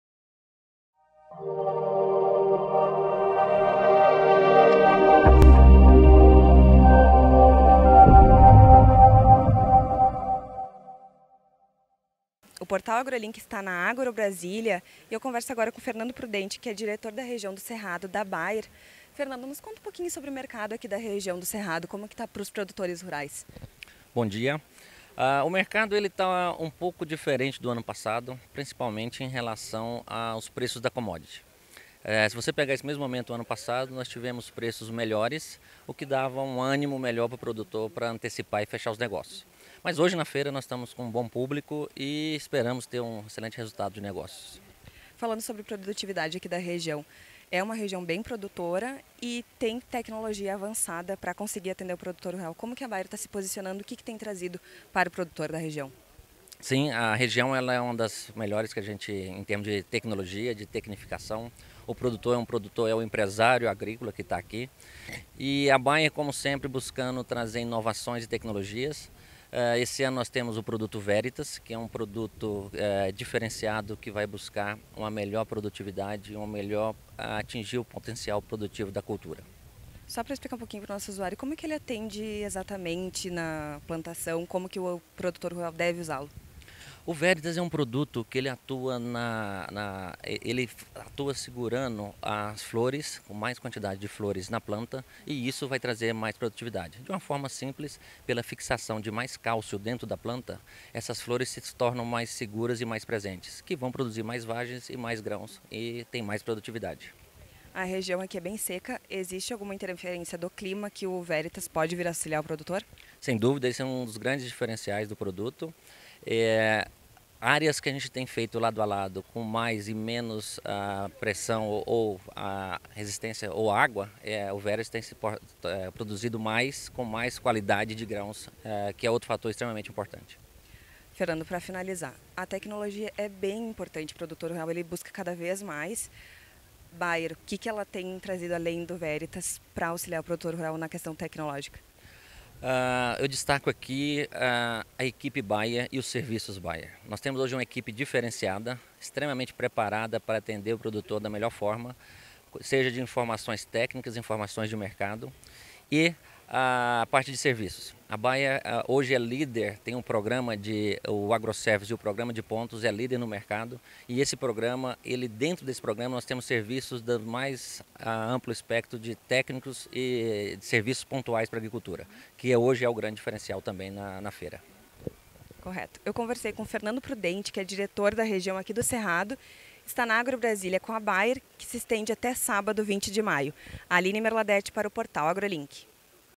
AgroBrasilia - 10 anos - Bayer
Durante a AgroBrasília, a Bayer falou sobre as tecnologias oferecidas pela empresa para auxiliar o produtor rural a aumentar a sua produtividade.